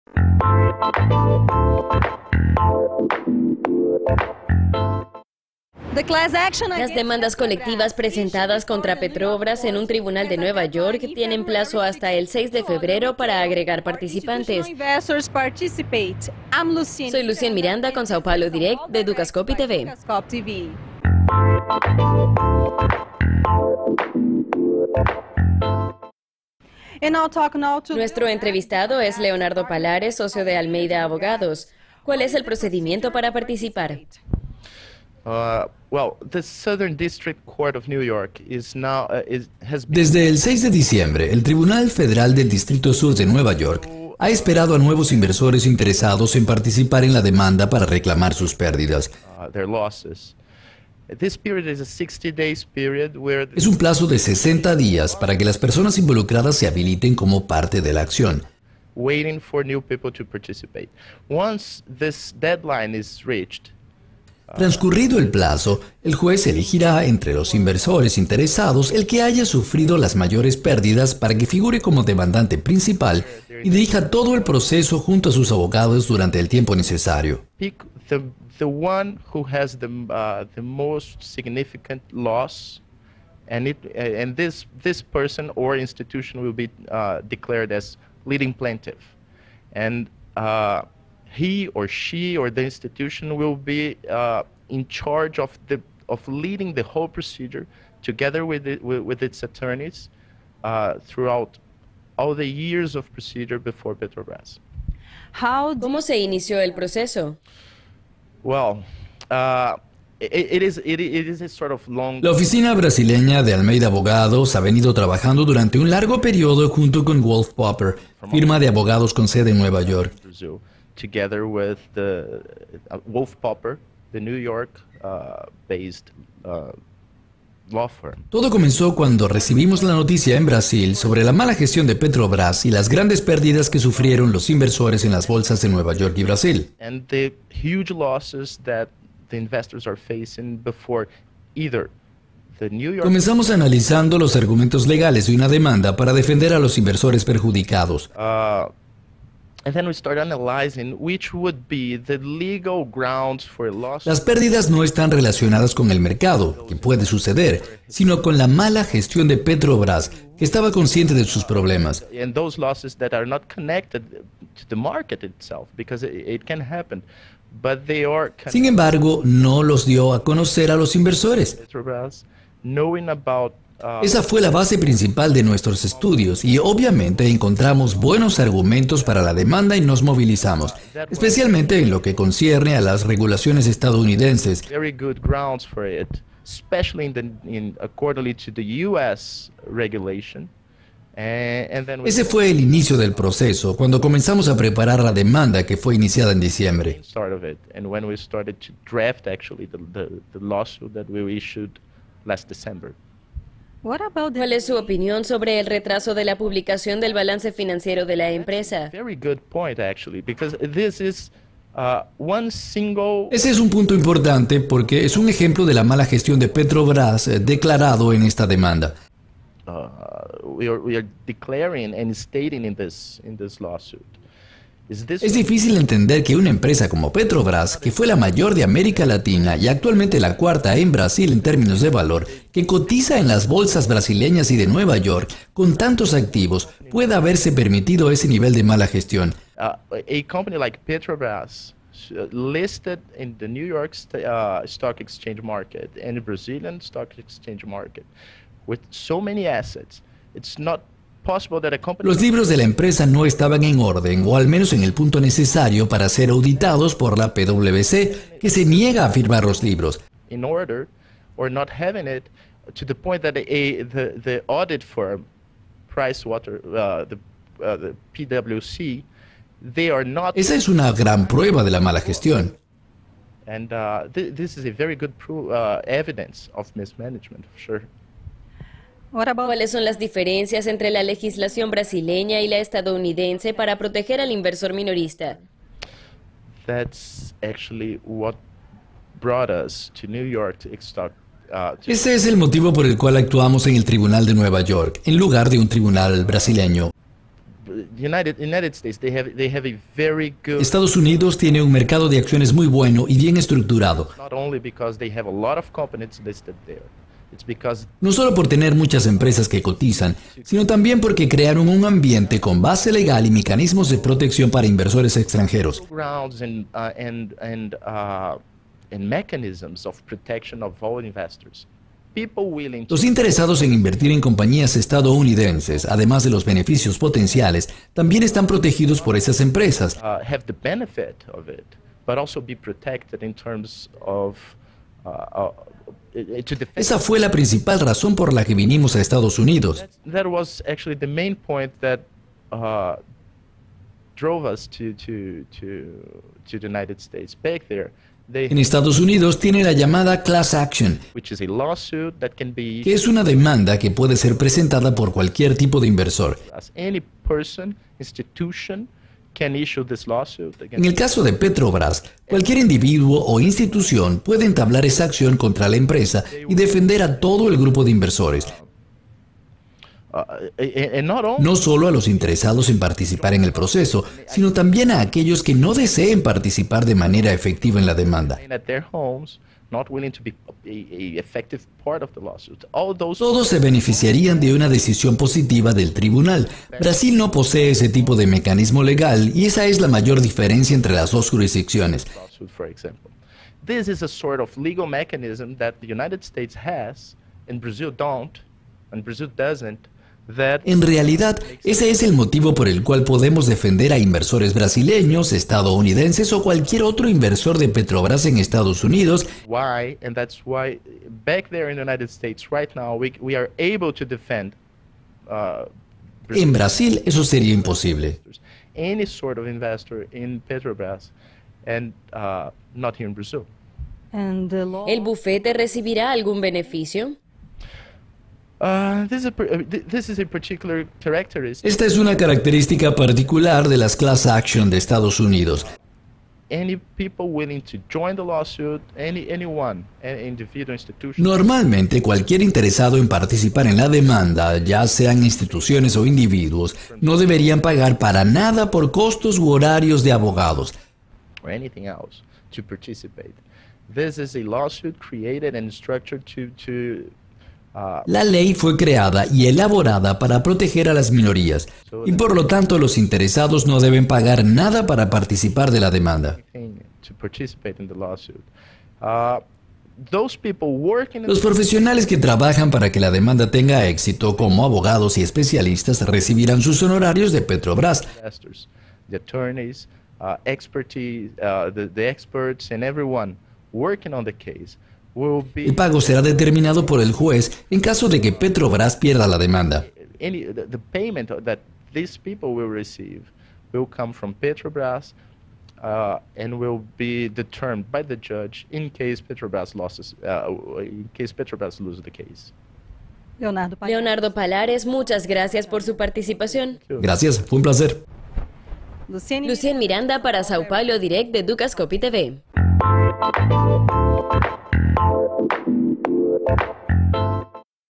Demanda contra Petrobras
Acciones colectivas contra Petrobras en EE.UU. Leonardo Palhares, abogado socio de Almeida Advogados

Puede mirar este video y los videos completos de archivo en la página de Dukascopy TV